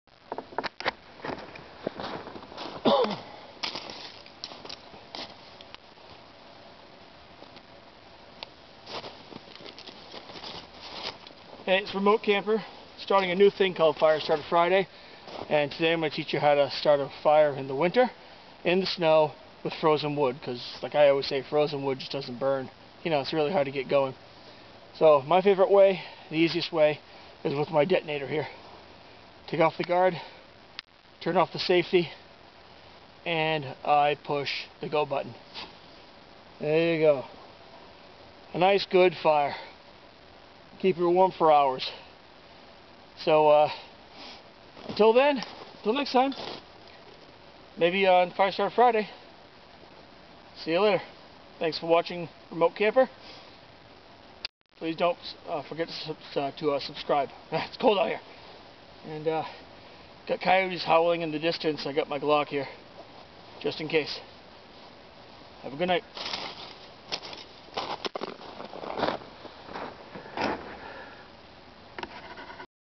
Firestarter Friday
Starting a fire in the winter is challenging.  The frozen wood is hard to get lit and keep going.
This is my favorite way.